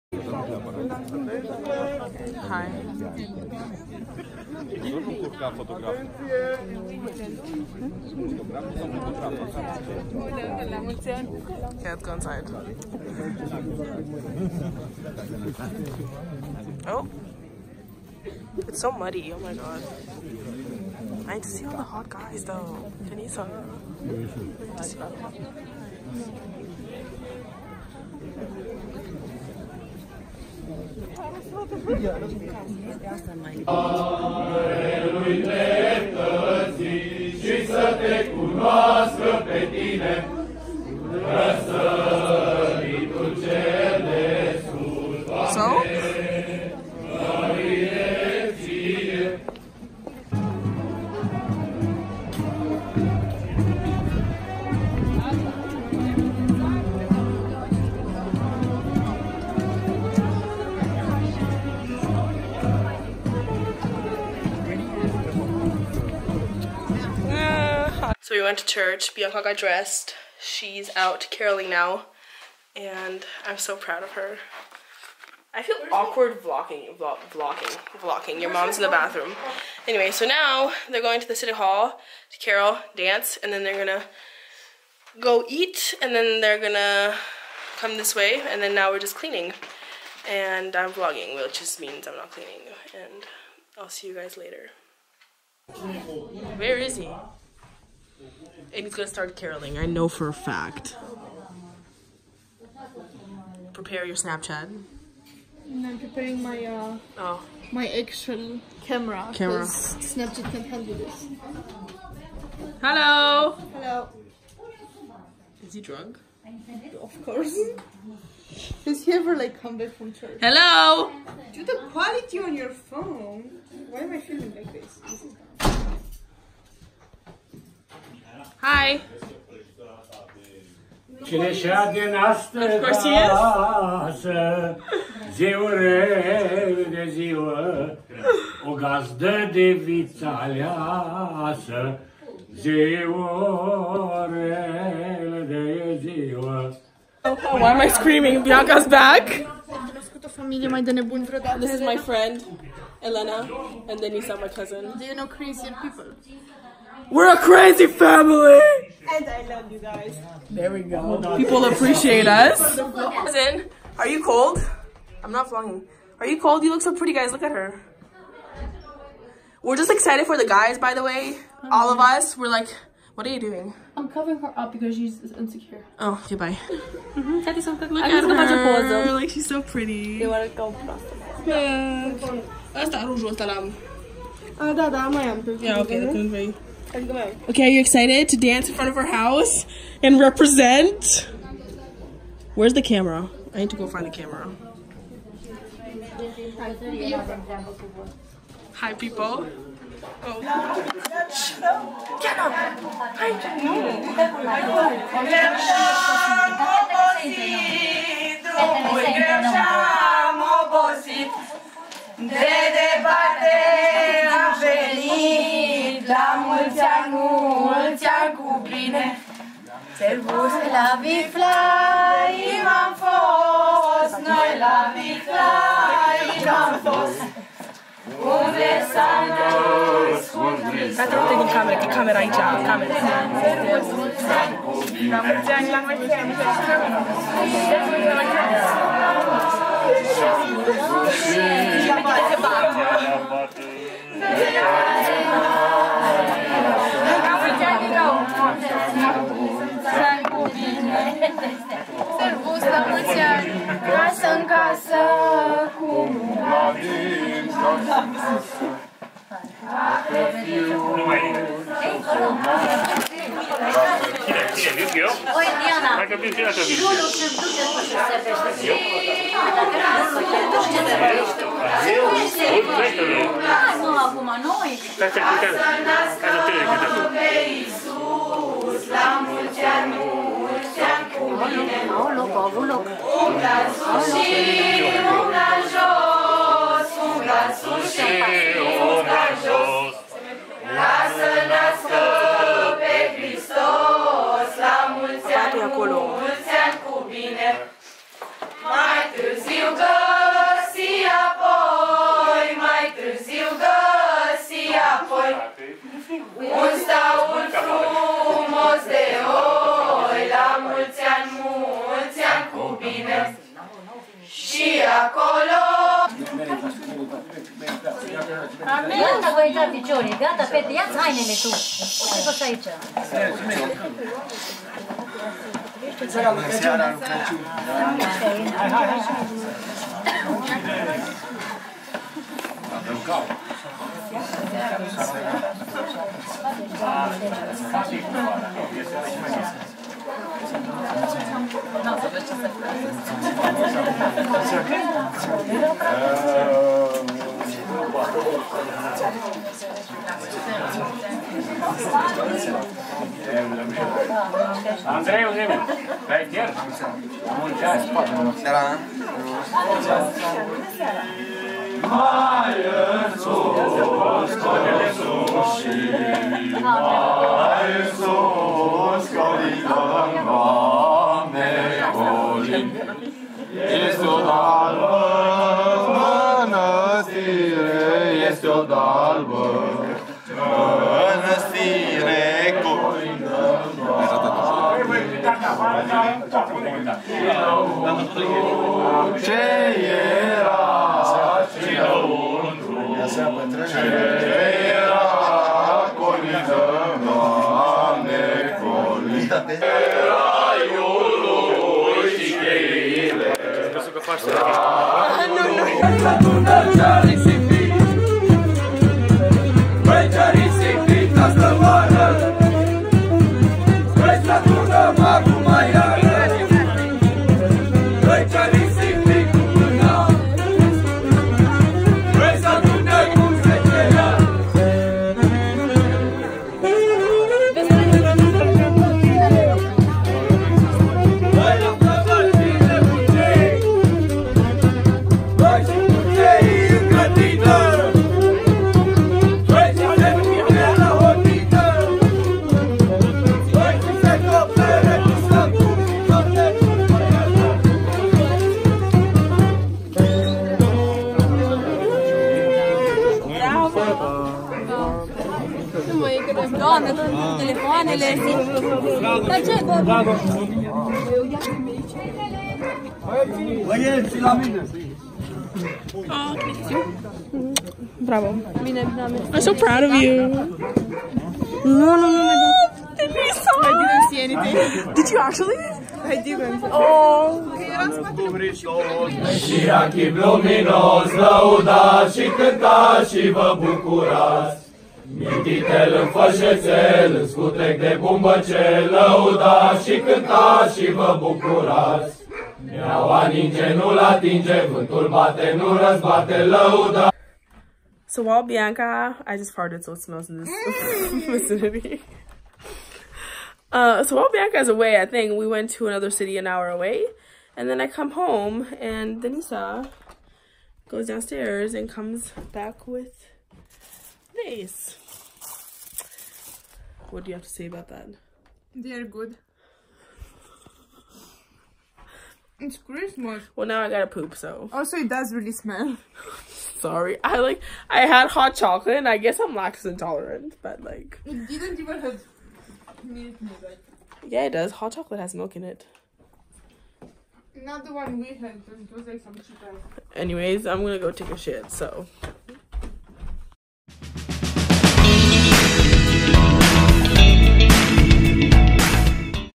Traditional Caroling in Romania
Bianca took part in the traditional caroling in our home town. We grew up watching people come at our house and carol and it was a privilege to see Bianca go and participate.

BIANCA: bianca.cantor
DIANA: dia2448